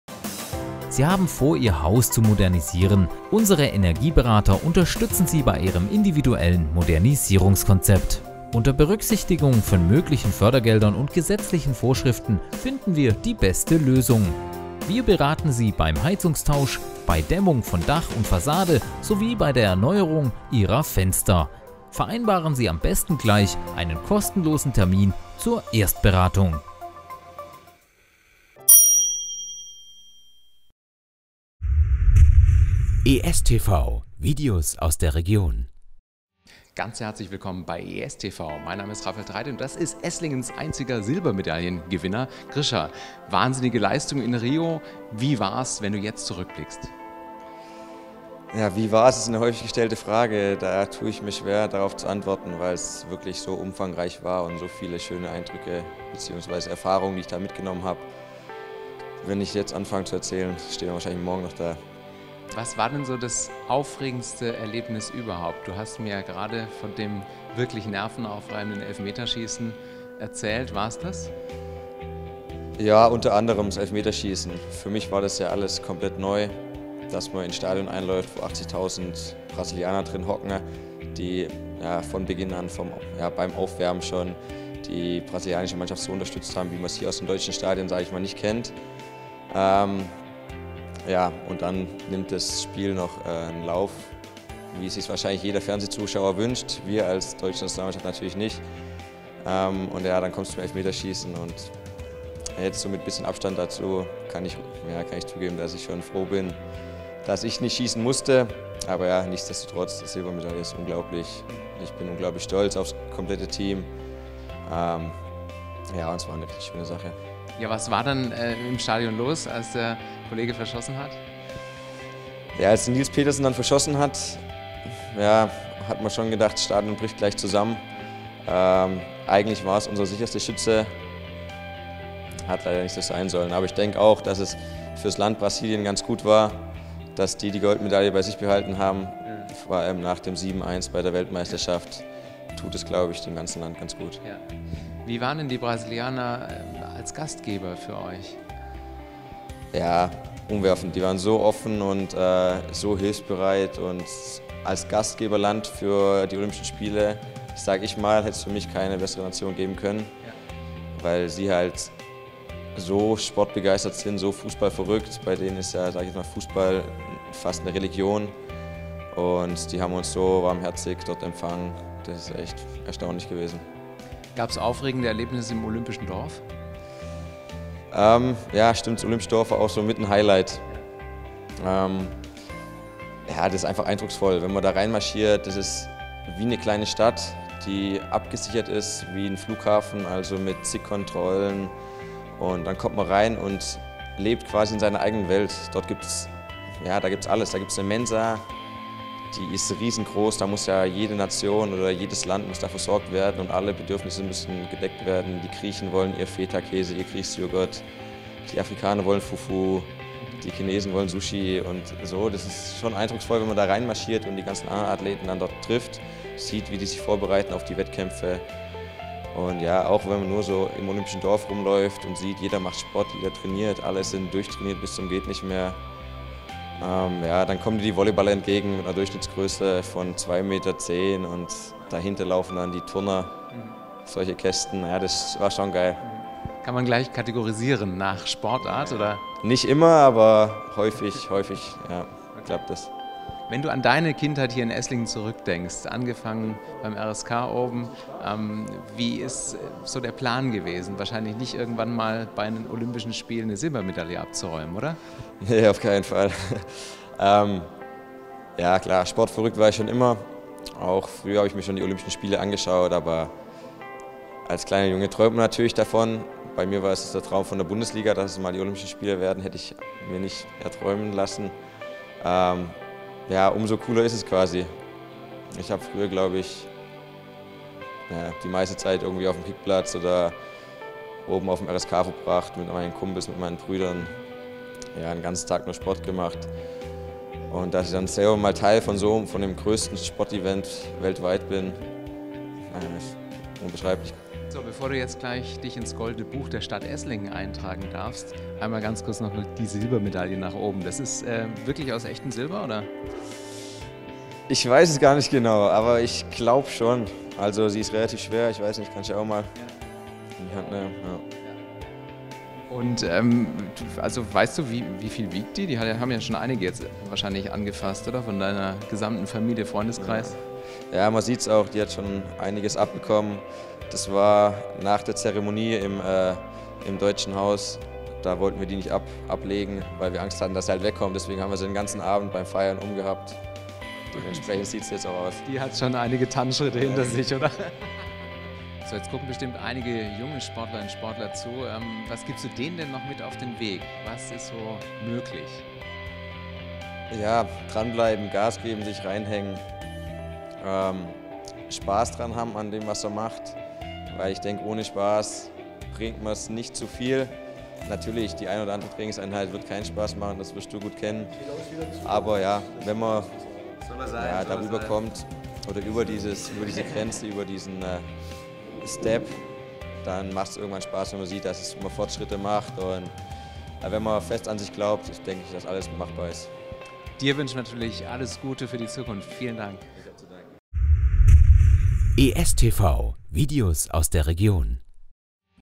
Interview mit Grischa Prömel: Ein Esslinger bei Olympia
Er lernte in Esslingen das Fußballspielen und stand vergangenes Jahr mit der deutschen Mannschaft im olympischen Finale gegen Brasilien und gewann dabei die Silbermedaille: Grischa Prömel.
Wir haben den sympatischen Kicker, der jetzt für den Karlsruher SC am Ball ist, getroffen.